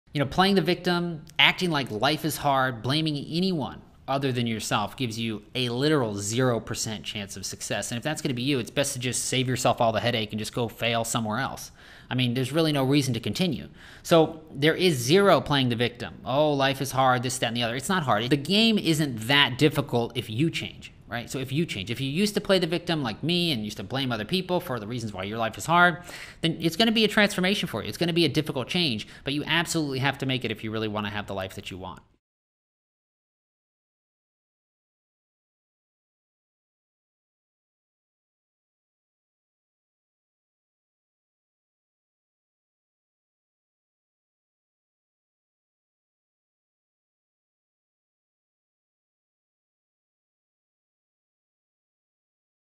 STOP Being the Victim!
When you play the victim and act like life is so hard that you have to blame others for everything then you will have ZERO chance of success. If this is you, then it's best that you quit or fail somewhere else. However, if YOU are willing to change and turn your mindset around then you will be able to see a significant transformation in your life AND your business.